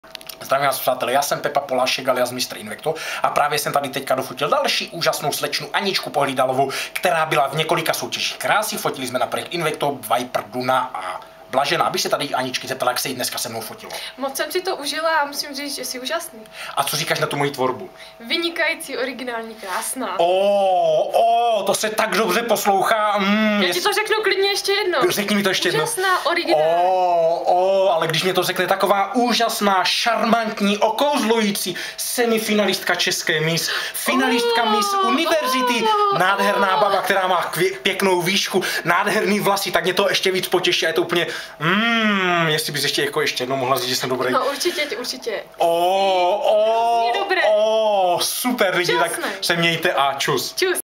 Anna Pohlídalová by mr. Invekto III...
Měl jsem fotit další úžasnou dívku, která byla v několika soutěžích krásy...